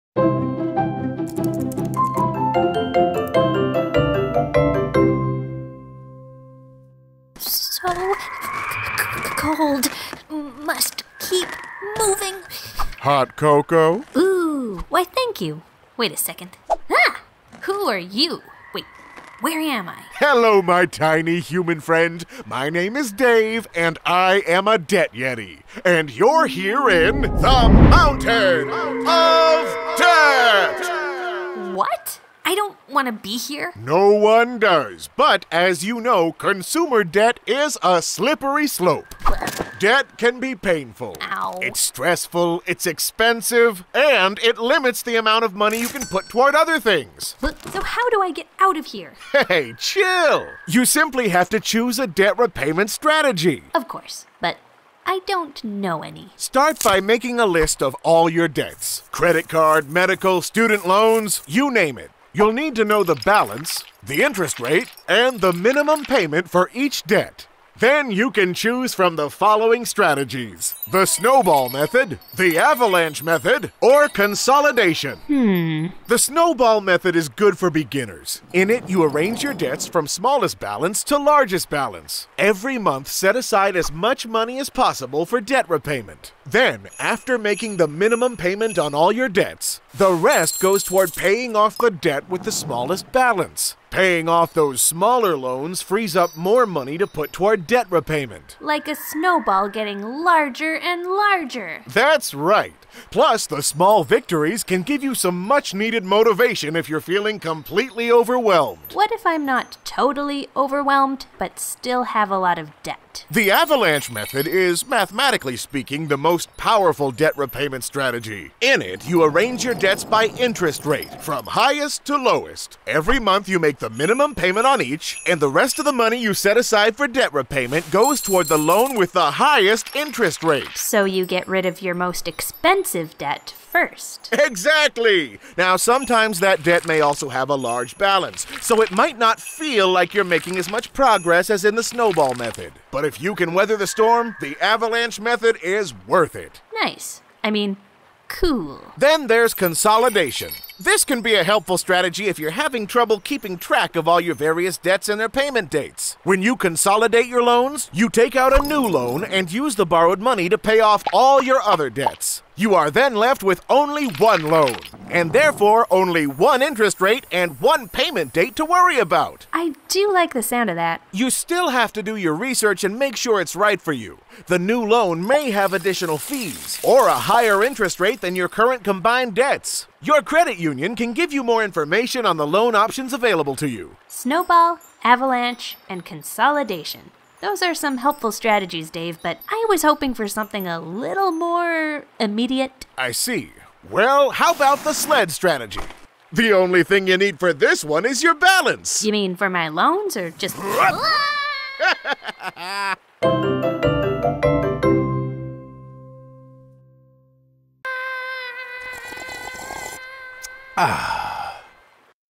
Strategies for Debt Repayment
Stuck in a mountain of debt? Debt is stressful, it's expensive and it limits the amount of money you can put towards your life goals. Find out three ways to repay that debt - snowball, avalanche, and consolidation.